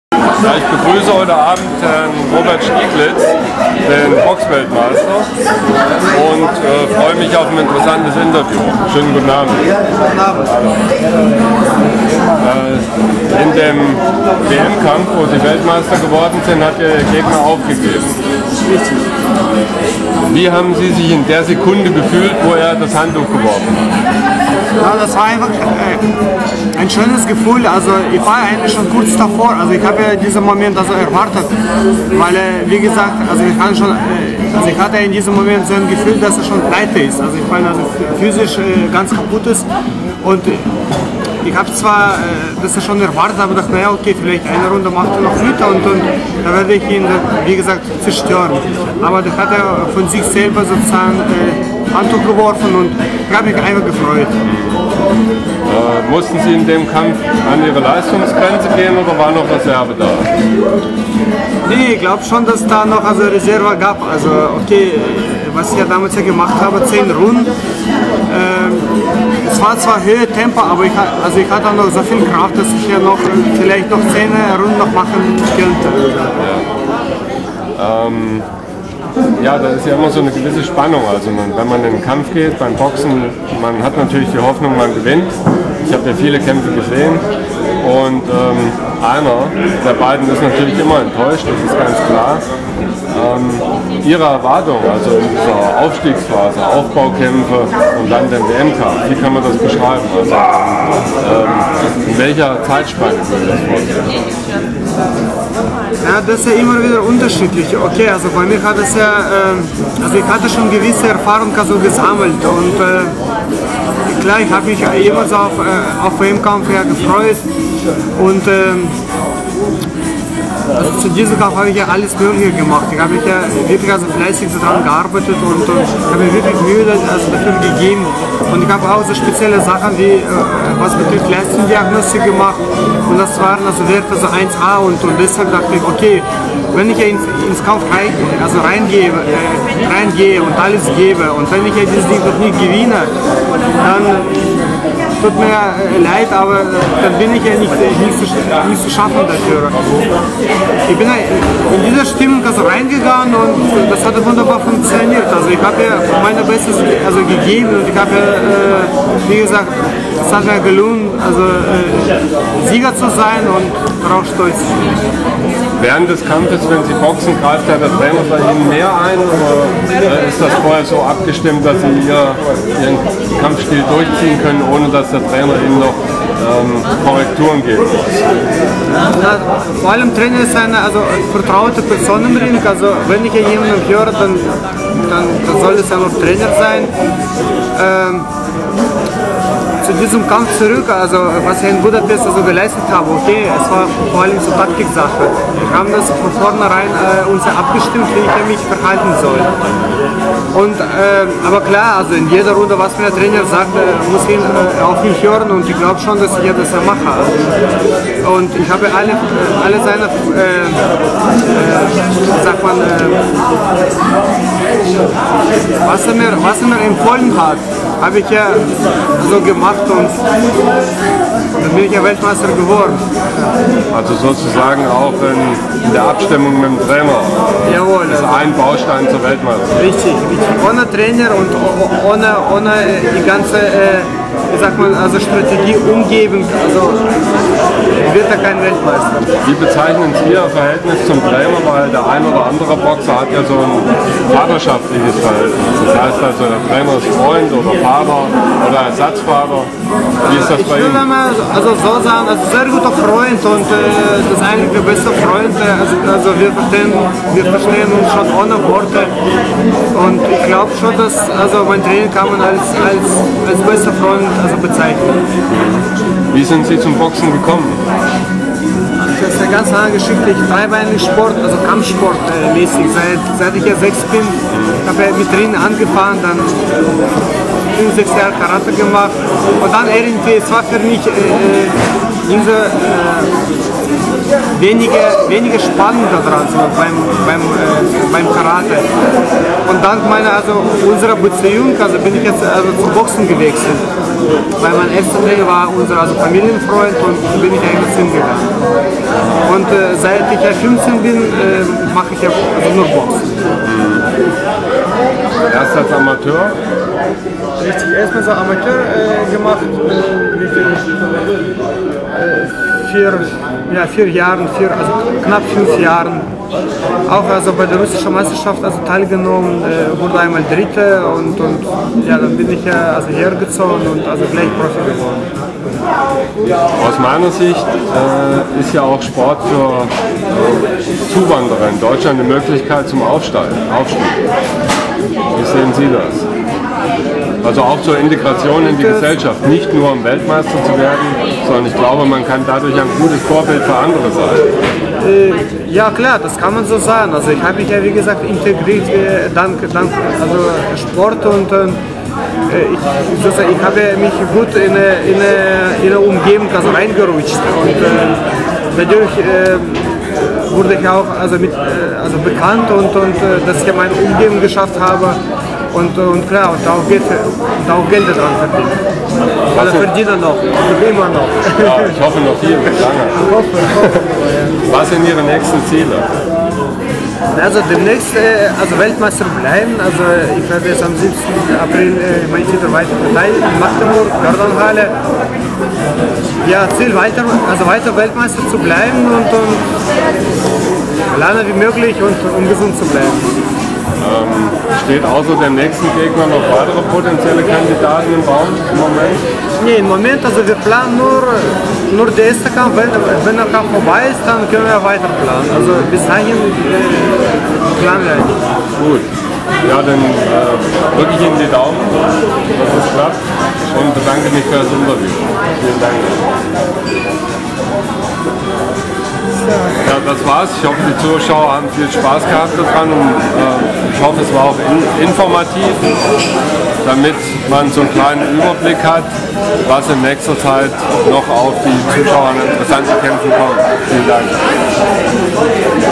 Robert Stieglitz - Interview Februar 2010
WBO-Weltmeister im Supermittelgewicht Robert Stieglitz erzählt in dem Interview über die bevorstehende Titelverteidigung gegen Eduard Gutknecht. Der Kampf findet am 17. April in der Magdeburger Bördelandhalle.